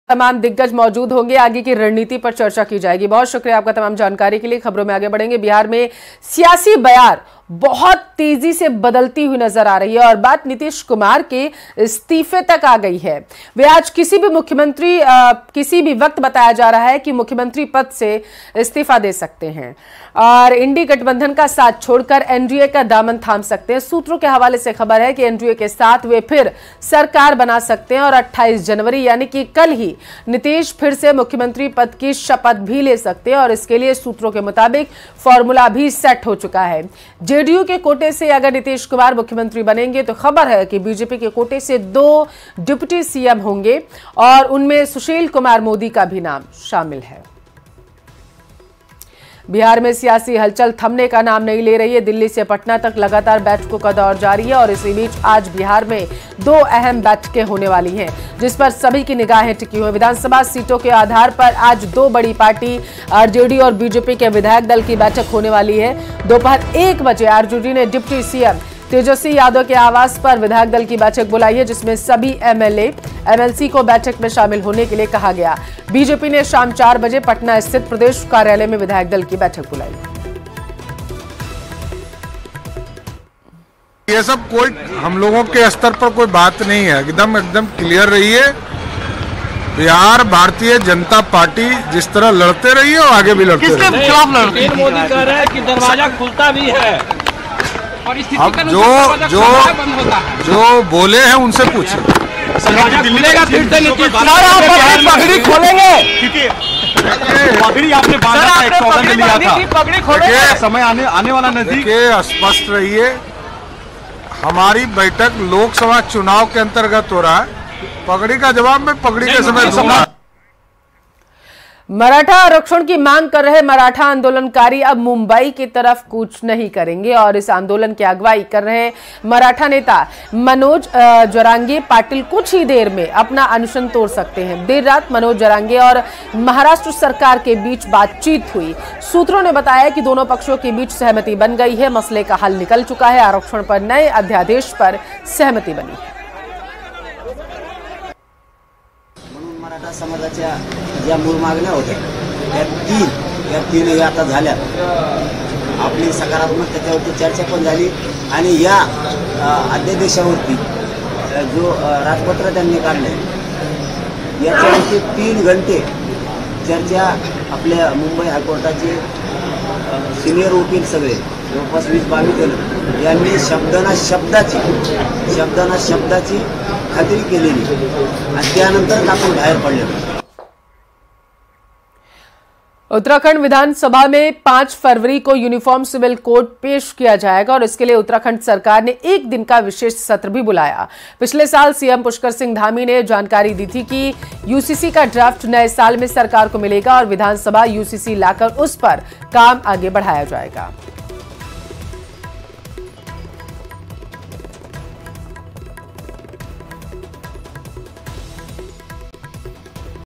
Bihar Political Crisis : बिहार की सियासत में हुआ कुछ ऐसा, सुन चौंक जाएंगे आप! | Breaking News | JDU
Bihar Political Crisis : बिहार की सियासत में हुआ कुछ ऐसा, सुन चौंक जाएंगे आप! | Breaking News | JDU | Tejashwi Yadav News

बिहार में सियासी हलचल तेज है और आने वाले 24 घंटे में काफी फेरबदल होने वाले हैं। बड़ी खबर ये है कि आज किसी भी वक्त बिहार में महागठबंधन के टूटने का ऐलान हो सकता है।